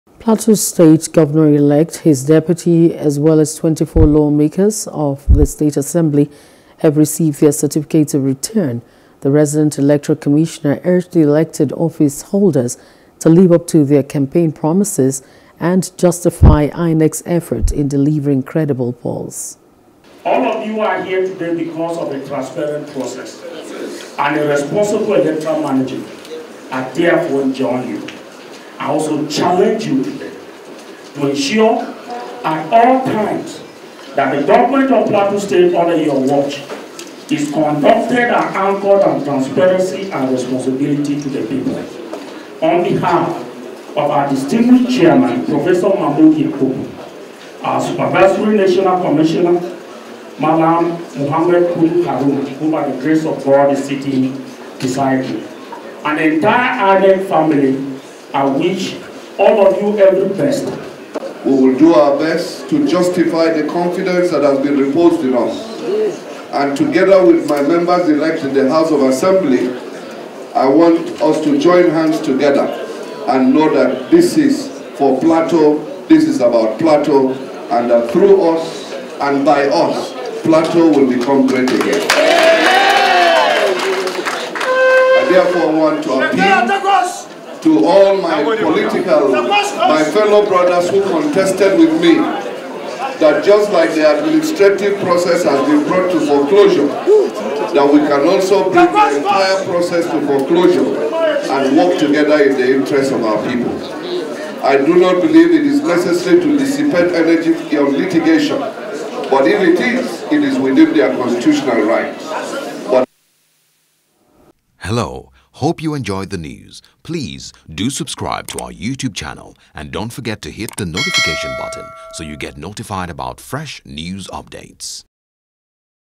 Gov/Assembly Elections: Plateau Gov-Elect Receives Cert. Of Return, Promises Dividends Of Democracy
Plateau governor-elect his deputy as well as 24 lawmakers of the Plateau State Assembly have received their certificate of return.